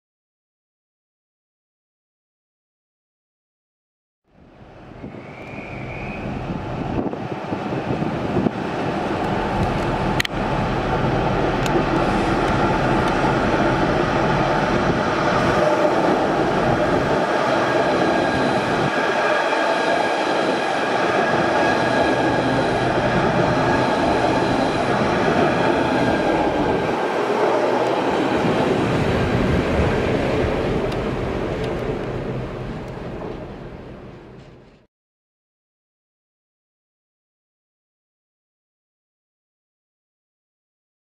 Southeastern High Speed Class 395 Javelin departs London St Pancras Intl *HD*
Southeastern High Speed Class 395 Javelin departs London St Pancras International operating 1J52 1512 St Pancras Intl to Margate.